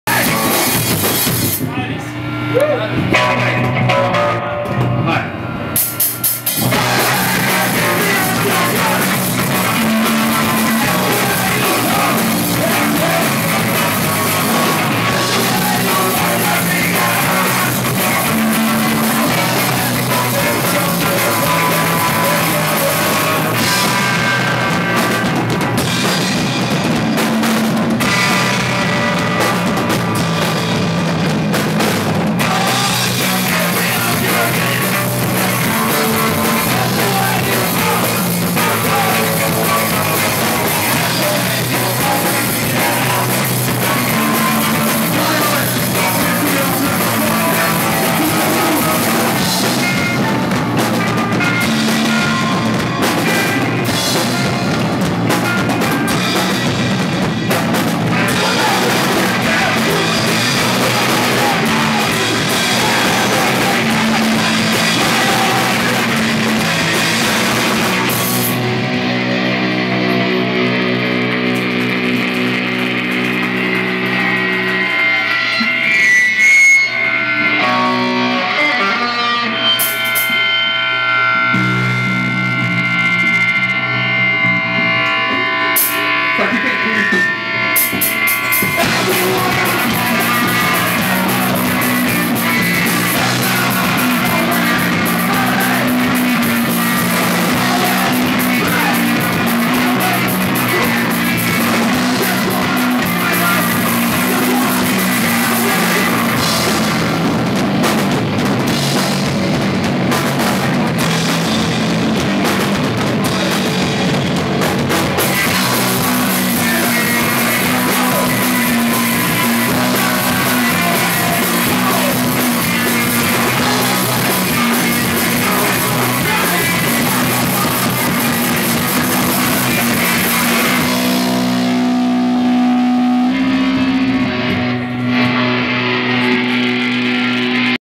Prokrastination Klan
Registro em vídeo de show do Prokrastination Klan em São Caetano do Sul. 28.10.12